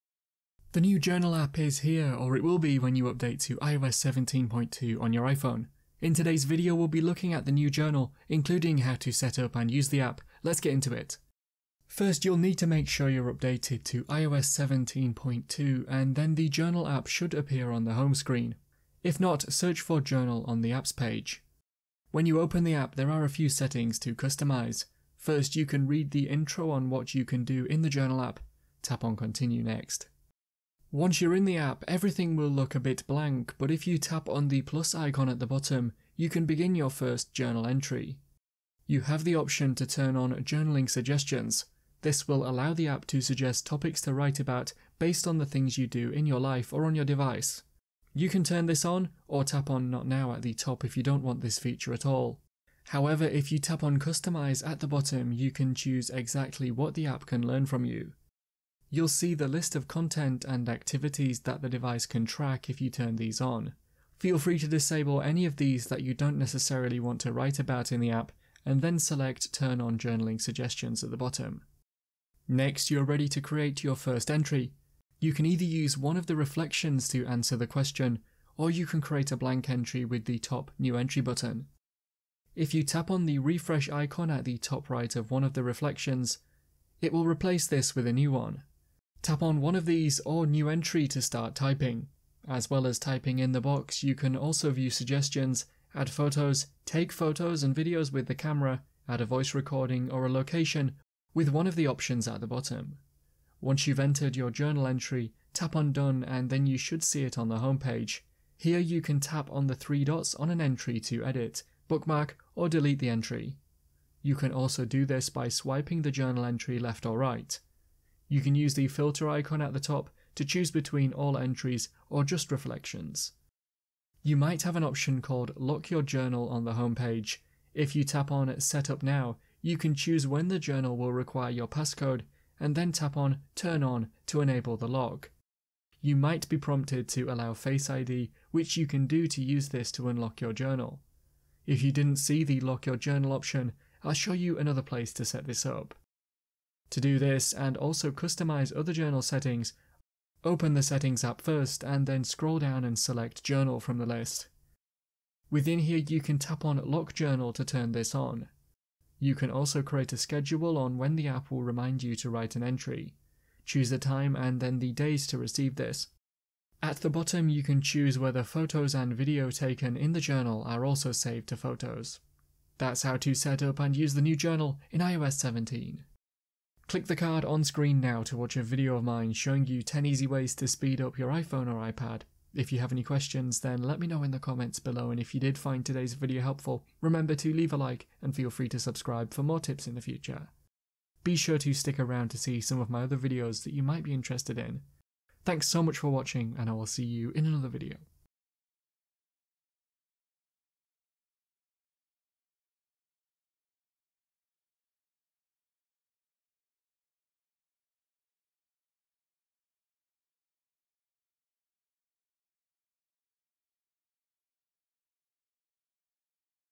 The JOURNAL APP is HERE! How to Set up & Use Journal on iPhone (iOS 17)
Want to know how to set up journal app on iPhone in iOS 17.2? This video will show you how to use journal app on iPhone in iOS 17. You might want to know how to get journal app on iPhone to use the new app’s features, where you can write journal entries based on the activities you do. Hopefully today’s video helps show you how to set up and how to use iOS 17 journal which is new in the iOS 17.2 update!